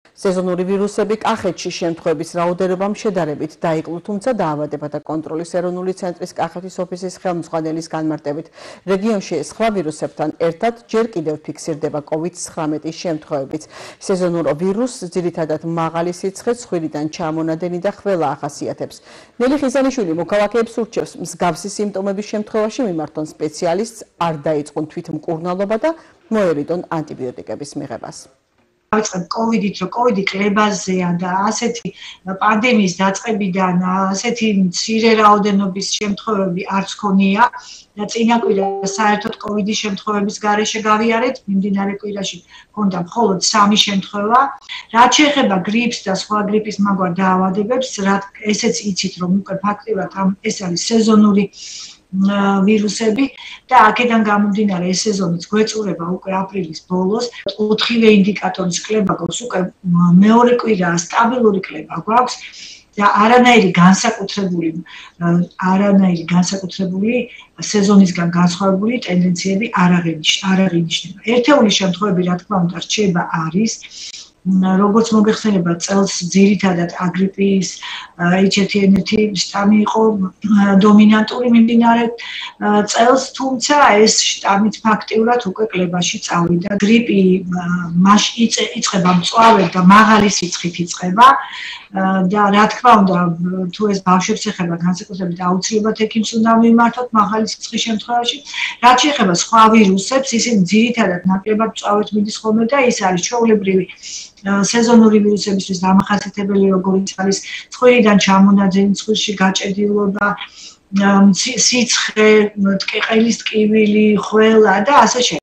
სეზონური ვირუსები კახეთში-რა მდგომარეობაა რეგიონში
სეზონური ვირუსები კახეთში. შემთხვევების რაოდენობამ შედარებით დაიკლო, თუმცა დაავადებათა კონტროლის ეროვნული ცენტრის კახეთის ოფისის ხელმძღვანელის განმარტებით, რეგიონში სხვა ვირუსებთან ერთად, ჯერ კიდევ ფიქსირდება კოვიდ 19-ის შემთხვევებიც. სეზონურ ვირუსს ძირითადად, მაღალი სიცხე, ცხვირიდან ჩამონადენი და ხველა ახასიათებს. ნელი ხიზანიშვილი მოქალაქეებს ურჩევს, მსგავსი სიმპტომების შემთხვევაში მიმართონ სპეციალისტს, არ დაიწყონ თვითმკურნალობა და მოერიდონ ანტიბიოტიკების მიღებას.